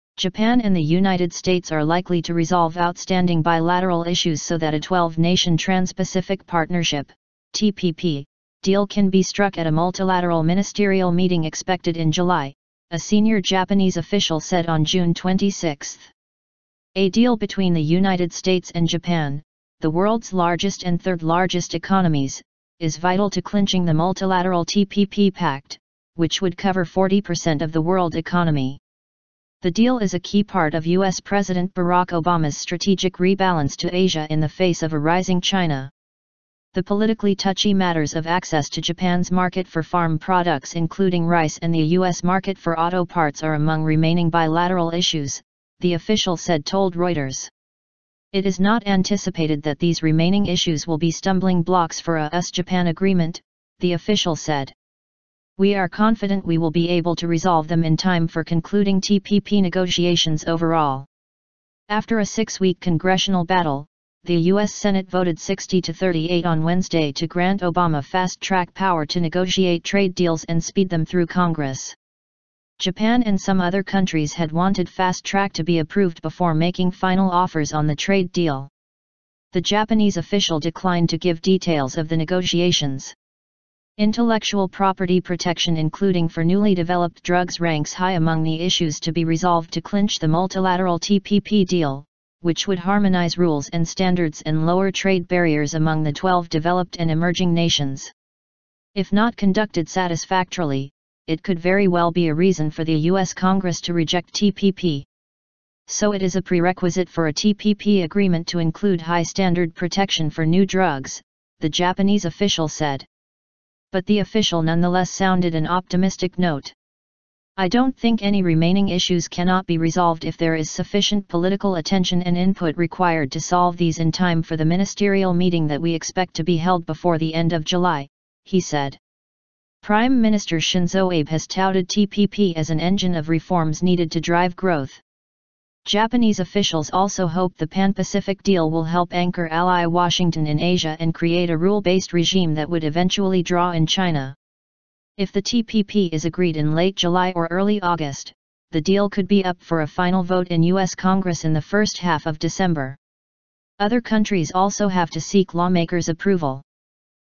Japan, US can reach agreement for July pan-Pacific trade deal: Japan official
Japan and the United States are likely to resolve outstanding bilateral issues so that a 12-nation Trans-Pacific Partnership (TPP) deal can be struck at a multilateral ministerial meeting expected in July, a senior Japanese official said on June 26.

A deal between the United States and Japan, the world's largest and third-largest economies, is vital to clinching the multilateral TPP pact, which would cover 40 percent of the world economy. The deal is a key part of US President Barack Obama's strategic rebalance to Asia in the face of a rising China.

The politically touchy matters of access to Japan's market for farm products including rice and the US market for auto parts are among remaining bilateral issues, the official said told Reuters.

"It is not anticipated that these remaining issues will be stumbling blocks for a US-Japan agreement," the official said.

"We are confident we will be able to resolve them in time for concluding TPP negotiations overall."

After a six-week Congressional battle, the US Senate voted 60 to 38 on Wednesday to grant Obama "fast-track" power to negotiate trade deals and speed them through Congress.

Japan and some other countries had wanted fast-track to be approved before making final offers on the trade deal. The Japanese official declined to give details of the negotiations.

Intellectual property protection including for newly-developed drugs ranks high among the issues to be resolved to clinch the multilateral TPP deal, which would harmonise rules and standards and lower trade barriers among the 12 developed and emerging nations.

"If not conducted satisfactorily, it could very well be a reason for the US Congress to reject TPP. So it is a prerequisite for a TPP agreement to include high standard protection for new drugs," the Japanese official said.

But the official nonetheless sounded an optimistic note.

"I don't think any remaining issues cannot be resolved if there is sufficient political attention and input required to solve these in time for the ministerial meeting that we expect to be held before the end of July," he said.

Prime Minister Shinzo Abe has touted TPP as an engine of reforms needed to drive growth. Japanese officials also hope the pan-Pacific deal will help anchor ally Washington in Asia and create a rule-based regime that would eventually draw in China.

If the TPP is agreed in late July or early August, the deal could be up for a final vote in US Congress in the first half of December. Other countries also have to seek lawmakers' approval.